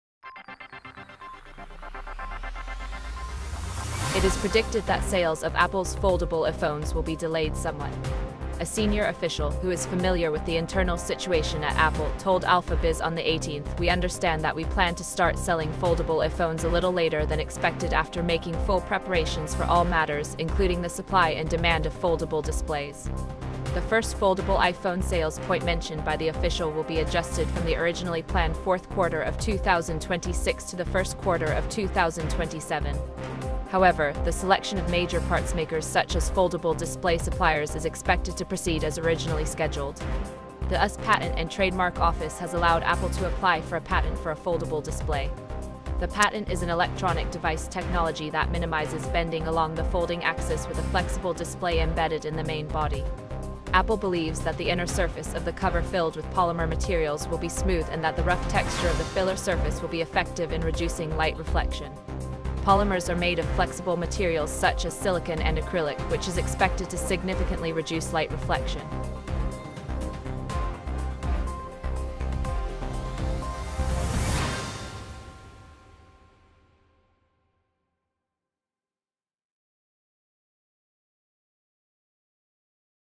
[Exclusive] Apple changes foldable iPhone sales date from late 2026 to early 2027
[Alpha Biz=(Chicago) Reporter Kim Jisun] It is predicted that sales of Apple's foldable iPhones will be delayed somewhat.

A senior official, who is familiar with the internal situation at Apple, told Alpha Biz on the 18th, "We understand that we plan to start selling foldable iPhones a little later than expected after making full preparations for all matters, including the supply and demand of foldable displays."

The first foldable iPhone sales point mentioned by the official will be adjusted from the originally planned fourth quarter of 2026 to the first quarter of 2027.

However, the selection of major parts makers such as foldable display suppliers is expected to proceed as originally scheduled.

The US Patent and Trademark Office has allowed Apple to apply for a patent for a foldable display.

The patent is an electronic device technology that minimizes bending along the folding axis with a flexible display embedded in the main body.

Apple believes that the inner surface of the cover filled with polymer materials will be smooth and that the rough texture of the filler surface will be effective in reducing light reflection.

Polymers are made of flexible materials such as silicon and acrylic, which is expected to significantly reduce light reflection.